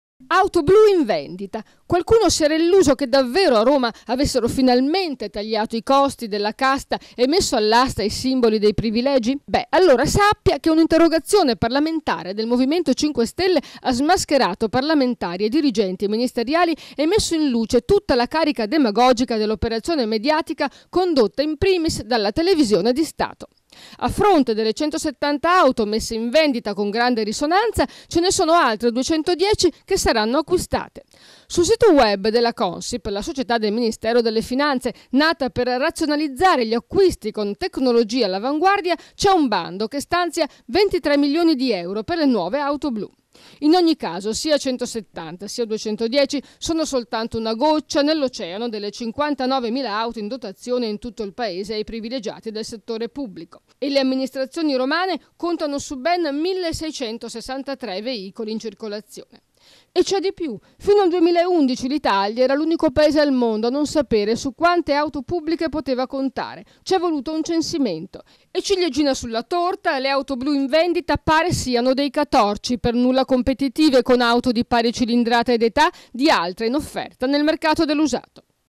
Impertinente: il business delle auto blu
A proposito di risparmio: ha avuto molto eco la vendita all'asta di 170 auto blu, ed è sorto l'equivoco tra molti telespettatori, che non si trattava di tagli ai privilegi, ma solo di sostituzione di auto  molto usate